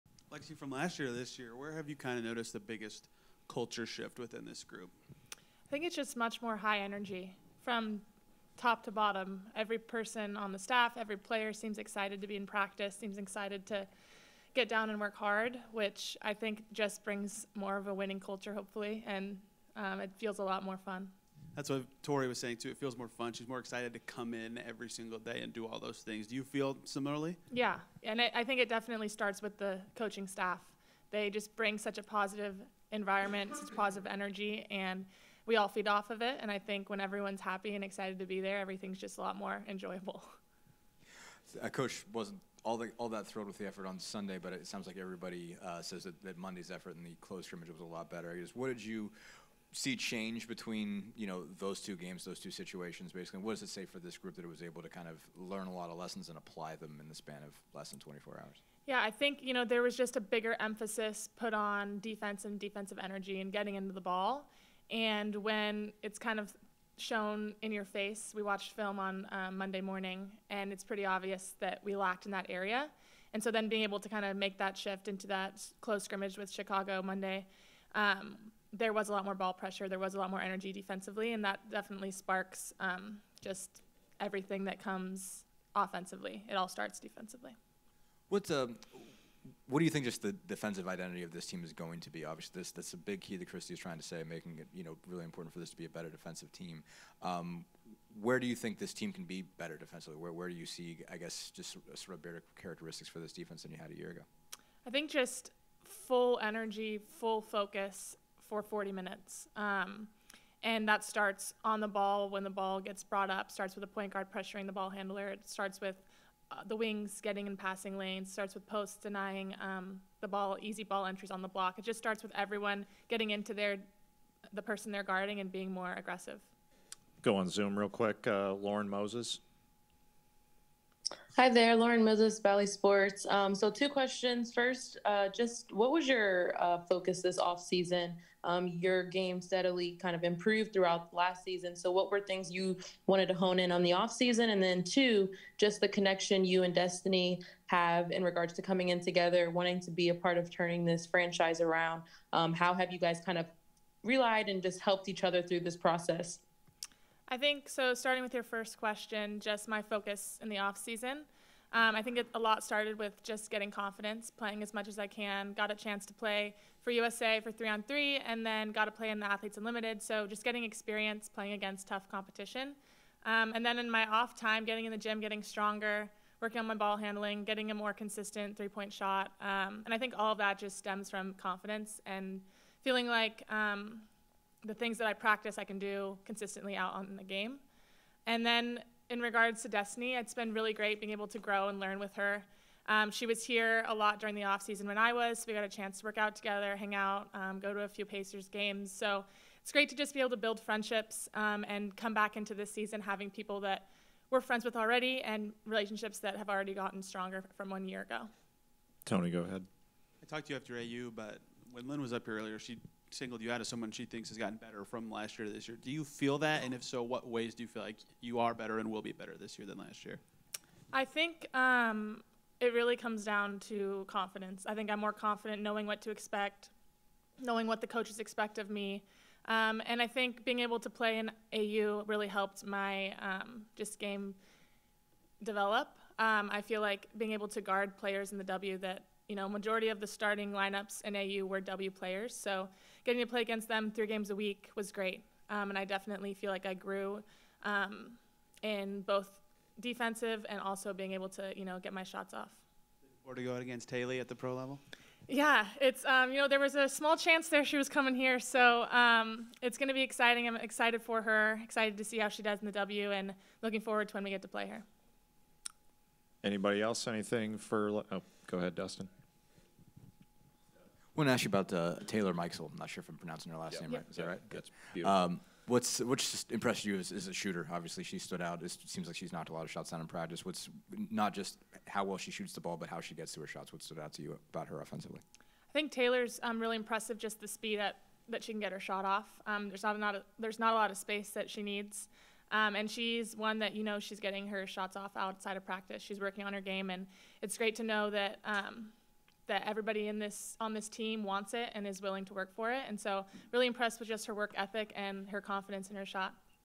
Lexie Hull Media Availability (May 10, 2023) | Indiana Fever
Fever guard Lexie Hull spoke with reporters at media day.